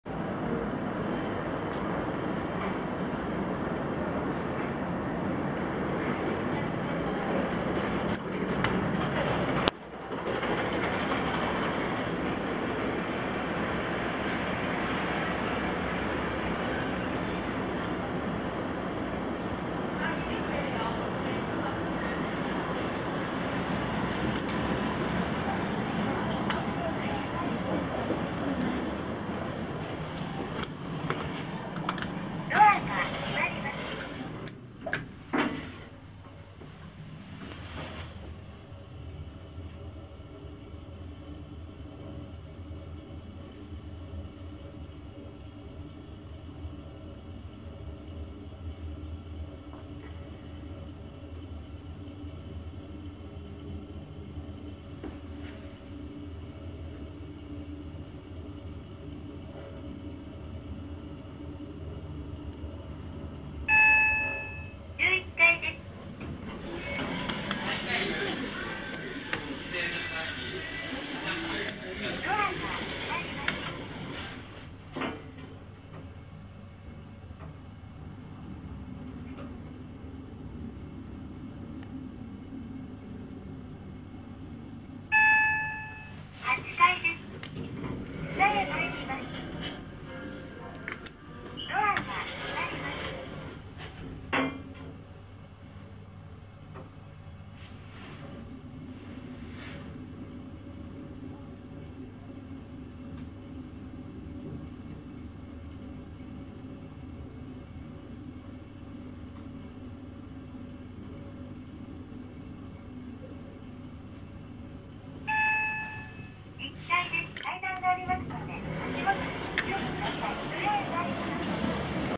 【乗場に段差】東京都中央区内某ビルのエレベータ― Part4
Shian’s Movie Theater.さんとのコラボ動画です！

メーカーはオーチス製です。1階の乗場に段差があるため、乗り場の高さが異なります。

用途：乗用
定員：11名
積載：750kg
設置年：不明
アナウンス：あり
到着音：あり
車椅子操作盤：あり（1機のみ）
エレベーター台数：2
機種：Gen2
方式：ロープ式